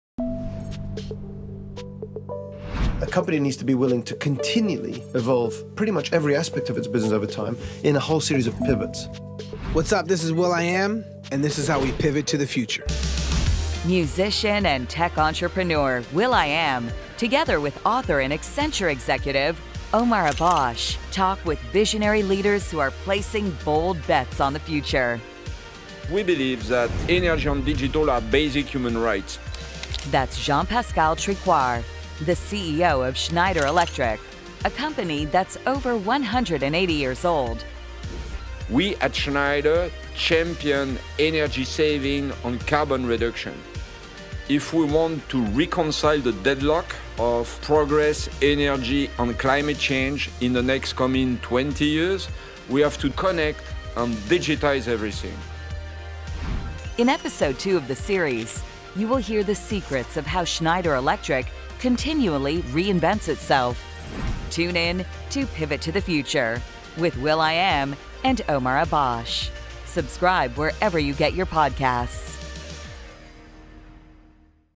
Pivot to the Future
Listen in as Will.i.am and Omar Abbosh speak with Schneider Electric Chairman & CEO Jean-Pascal Tricoire on how to have the courage to make a WisePivot.